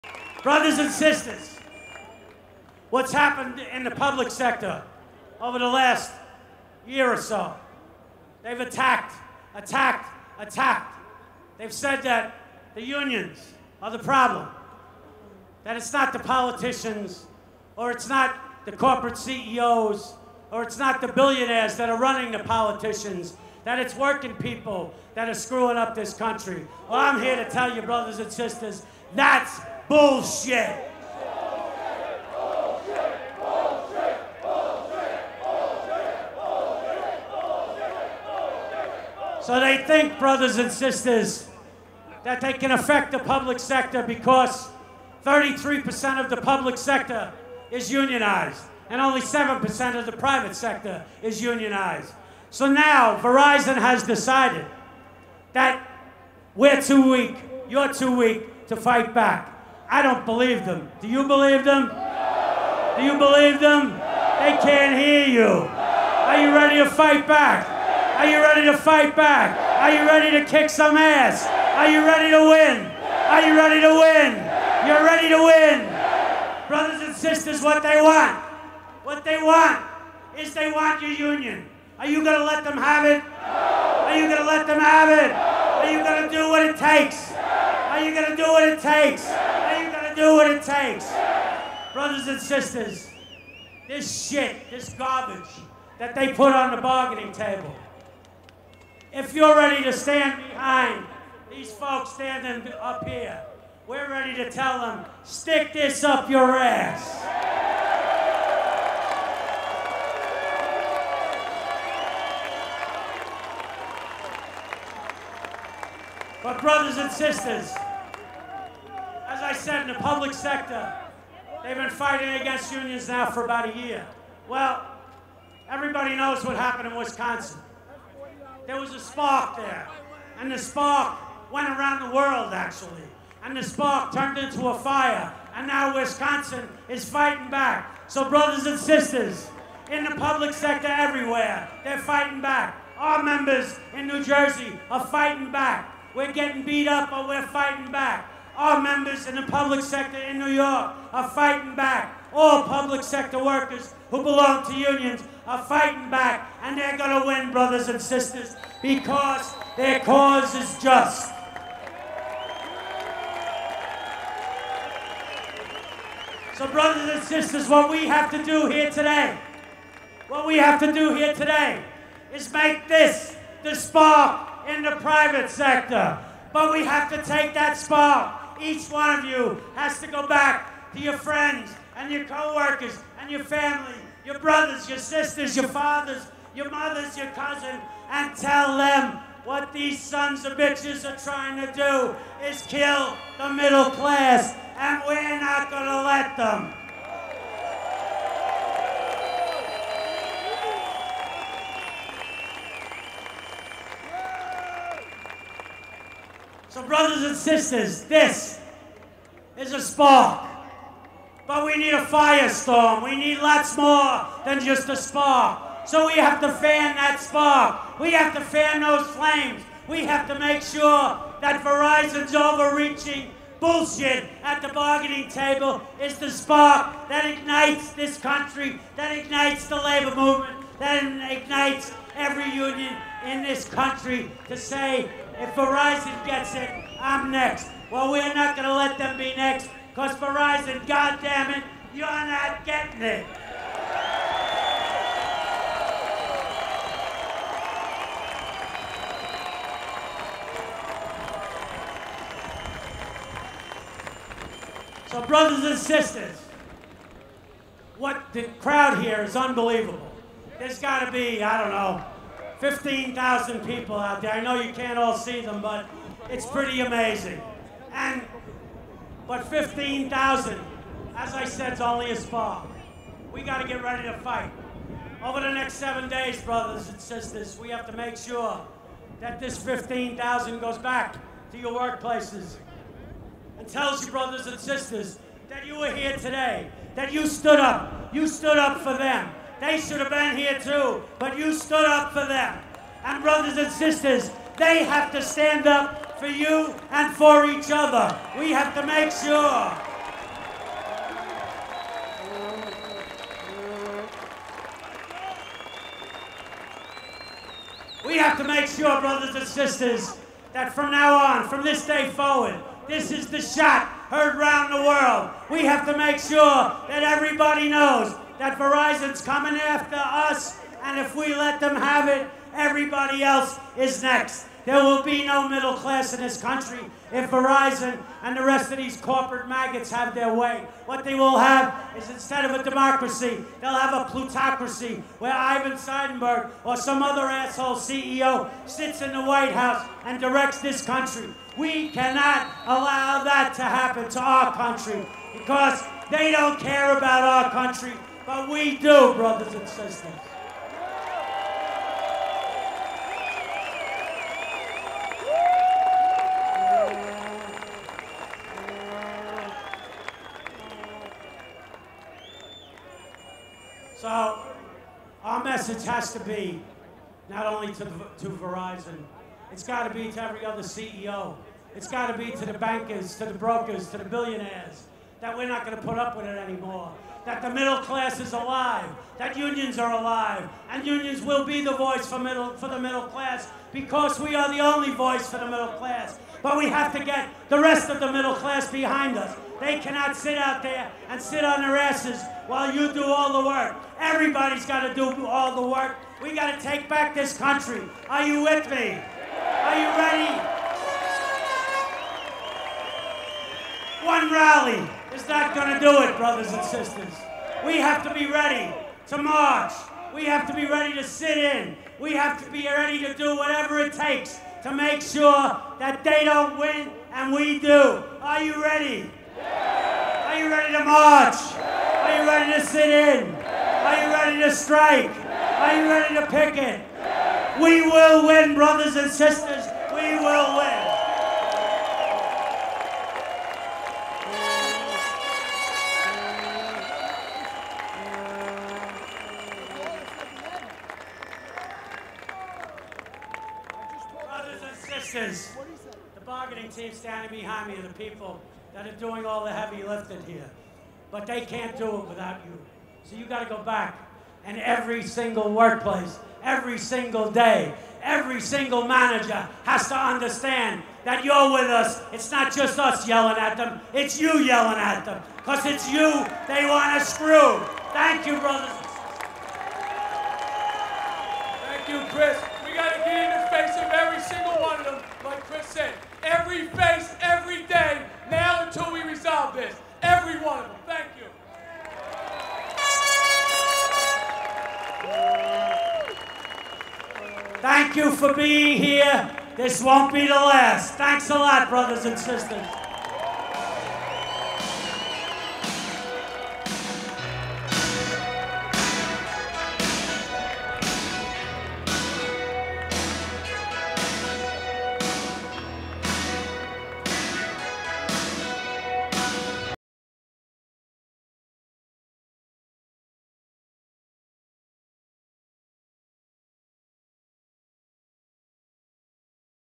Verizon Rally: CWA District 1 VP Chris Shelton (Part 2 of 2)
Over 15,000 Verizon employees took to the streets of New York City to demand good jobs and a fair contract. 7/30/2011.